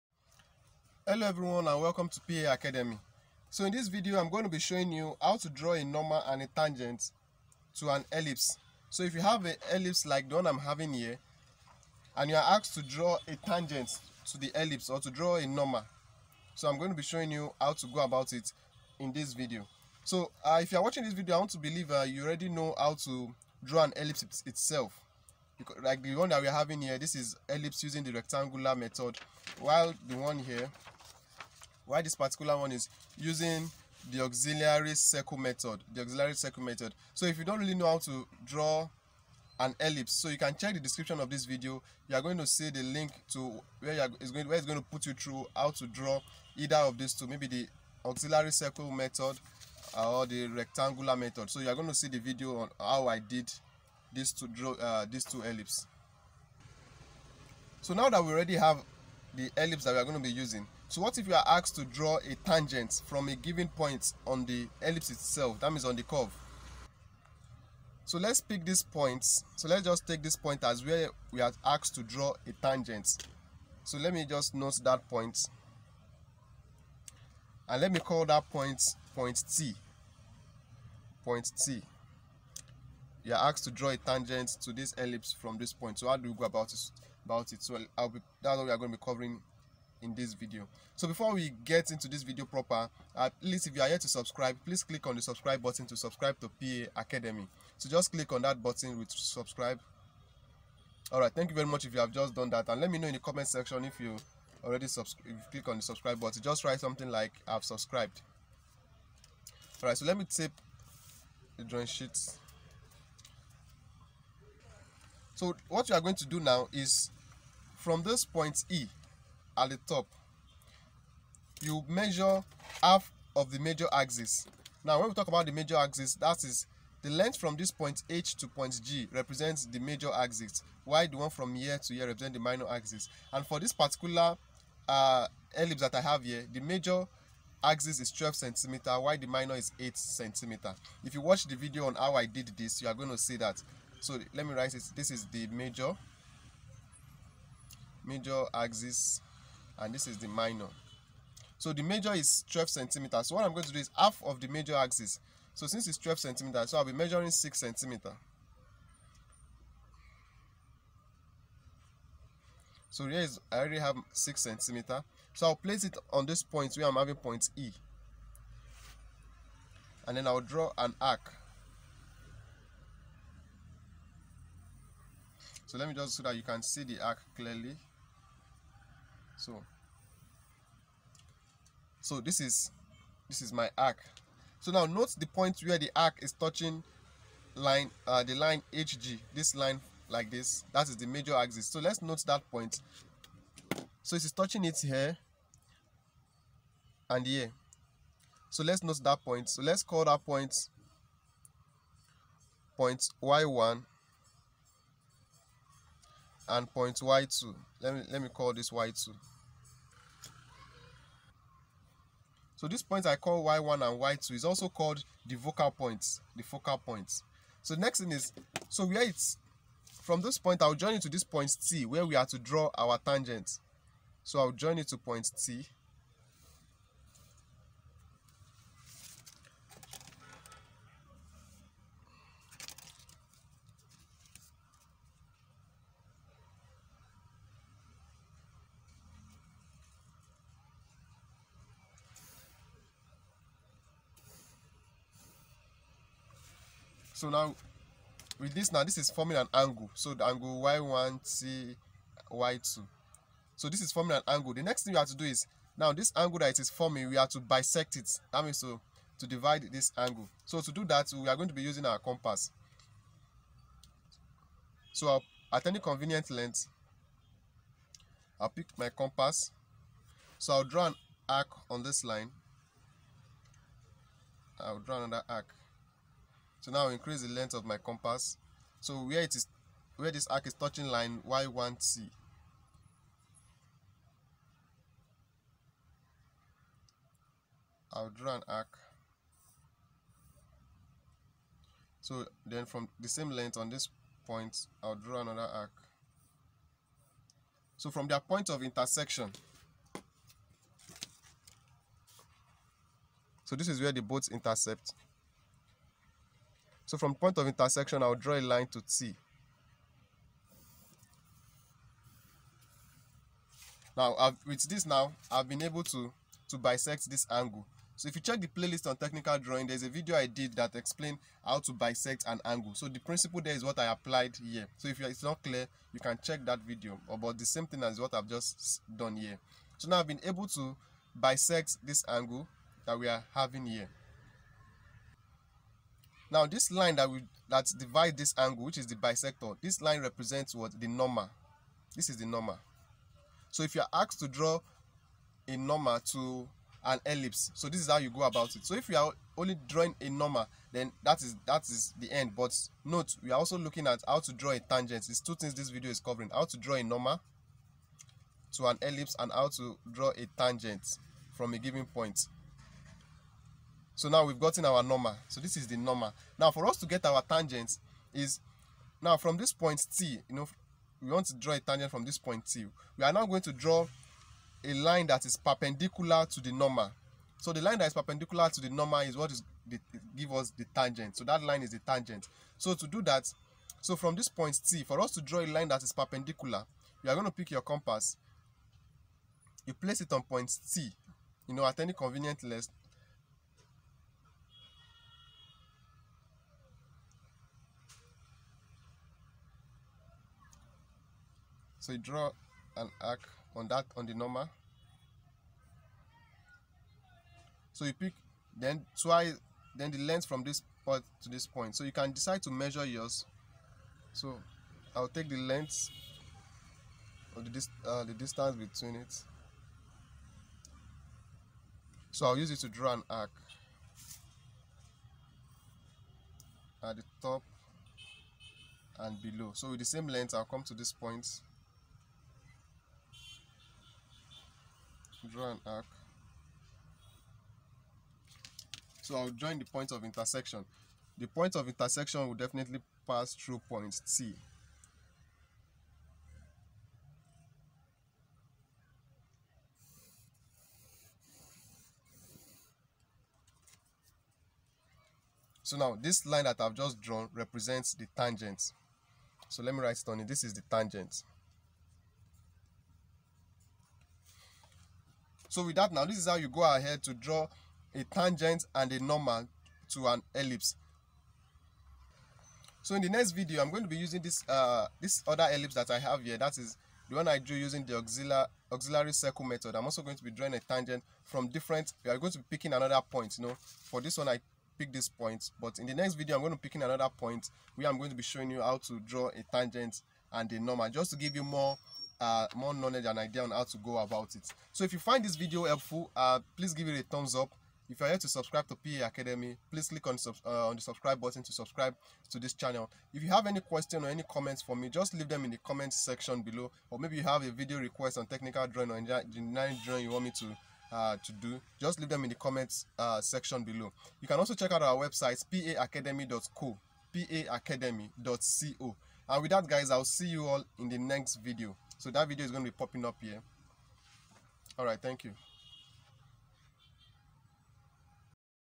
How to Construct a Tangent and Normal to an Ellipse. Example 1 | How to Draw a Tangent to an Ellipse
In this video, I will show you How to Construct a Tangent and Normal to an Ellipse. This is for example 1. The link to example 2 is below.

Thanks
PA Academy